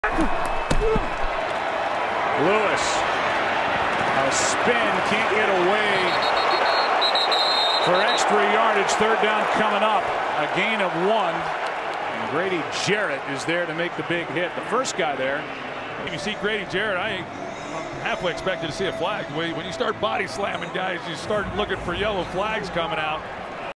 Grady Jarrett Body Slam on Dion Lewis (No Penalty)
Super Bowl LI Patriots vs Falcons 2017